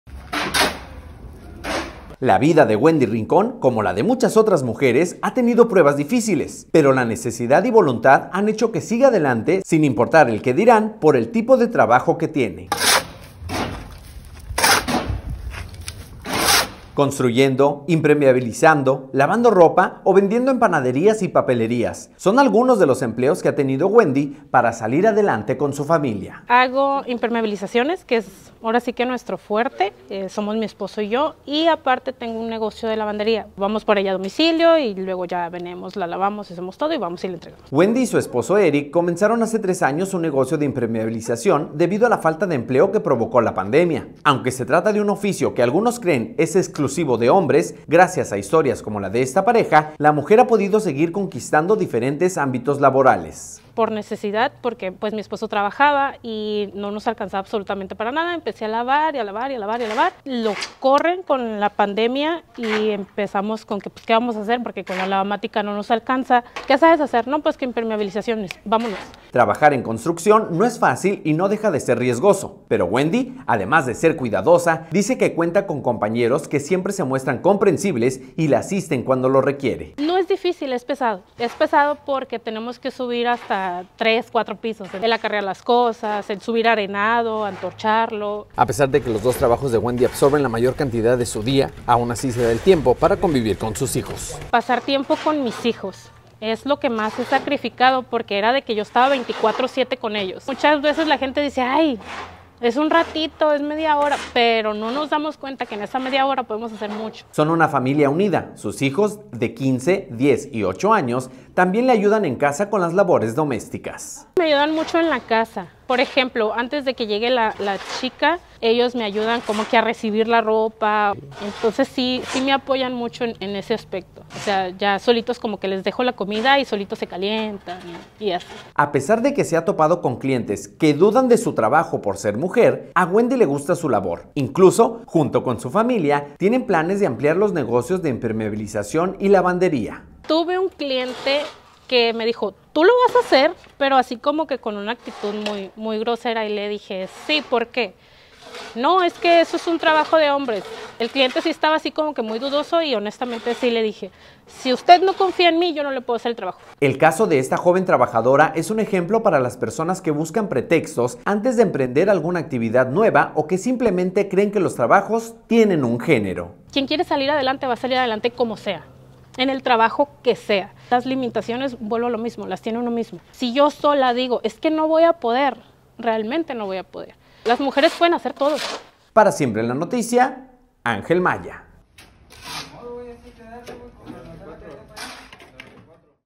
Mamá trabaja en construcción para sacar adelante a su familia
A Wendy no le importa el "qué dirán" y con su ejemplo nos demuestra que las mujeres pueden realizar lo que se propongan